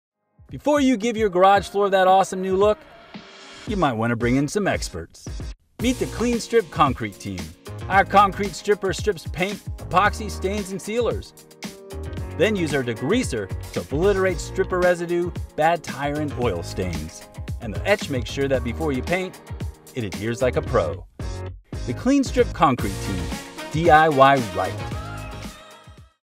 KS Concrete Collection
Are you ready to tackle tough concrete surfaces? Klean Strip has the perfect solution for every step of the way! Whether you're prepping a garage floor, driveway, or patio, our Klean Strip Concrete products make it easy to clean, strip, and etch your concrete for a flawless finish.